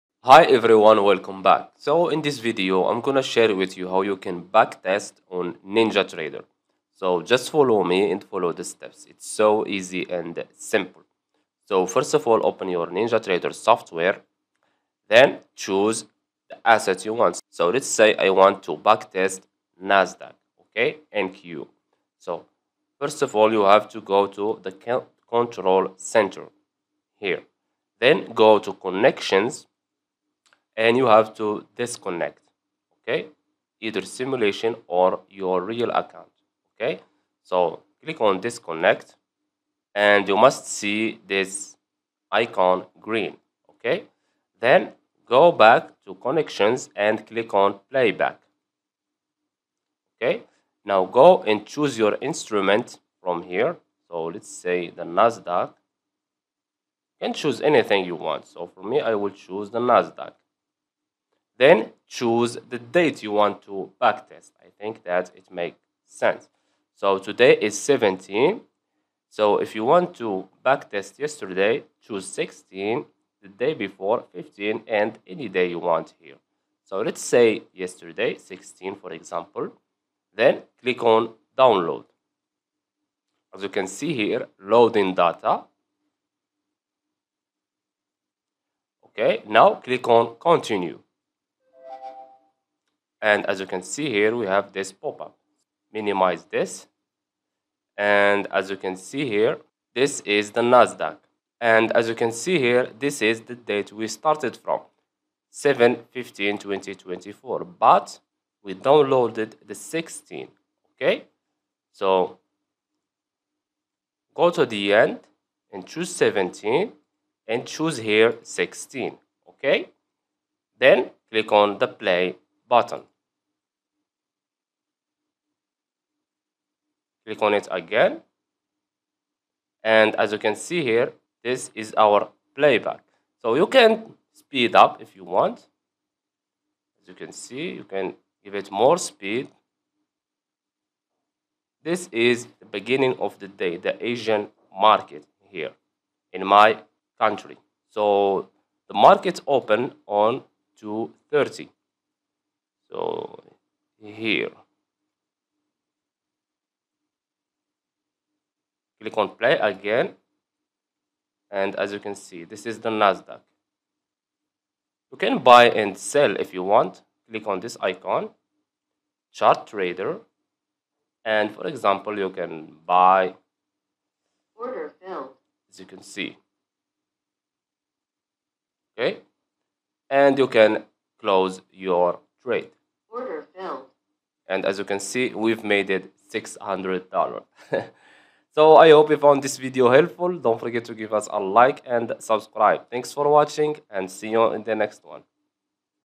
How to Backtest in NinjaTrader 8 | How to Use Market Playback
In this video, we'll demonstrate how to backtest your trading strategies using the Market Playback feature in NinjaTrader 8. Learn how to simulate past market conditions, test your strategies, and refine your approach without risking real capital. Follow our step-by-step guide to effectively utilize Market Playback for more informed trading decisions and improved performance.